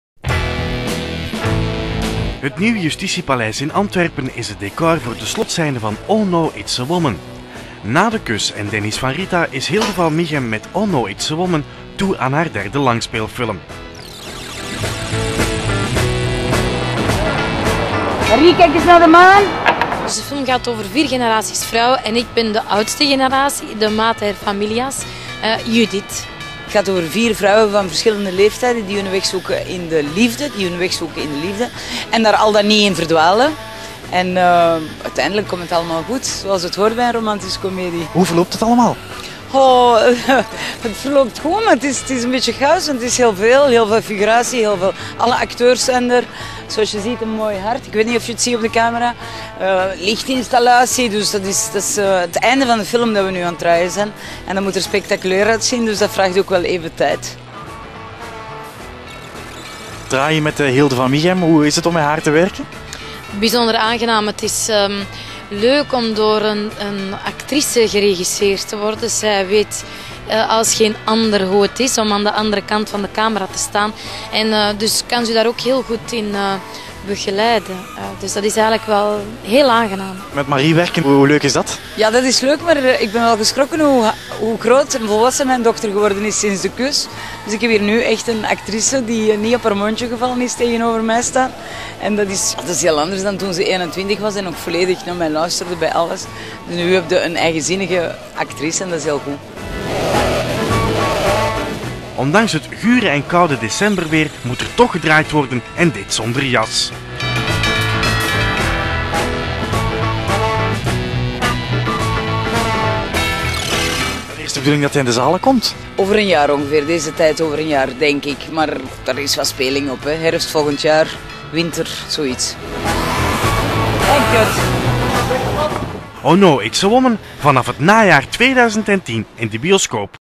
Op de set van Oh No! It's a woman
We trokken naar de set van Oh No! Its a Woman.
Het nieuwe jusititie paleis in Antwerpen is het decor voor de slotscene van Oh No! Its a Woman. Na de Kus en Dennis Van Rita is Hilde Van Mieghem met Oh No! Its a Woman toe aan het derde langspeelfilm.

Extra: De eigenwijze veertienjarige Eva gunt ons een speelse kijk in de muizenissen en amoureuze lotgevallen van zichzelf, haar zus, moeder en tante
doorheen de vier seizoenen. Een romantische feelgood komedie over een herkenbare, chaotisch-grappige familie, over vrouwen en mannen, moeders en dochters, trouw en ontrouw, en over
hoe een verpletterende kus in de regen of een gedeelde smoothie je leven kunnen
veranderen

Oh No! It's a woman is in de eerste plaats een speelse, betoverende, uitdagende én oprechte feelgoodfilm over de liefde. Hoe denken, voelen en praten vrouwen over liefde en alles wat daarmee samenhangt: romantiek, seks, vriendschap...?

Oh No! Its a Woman vanaf het najaar 2010 in de zaal.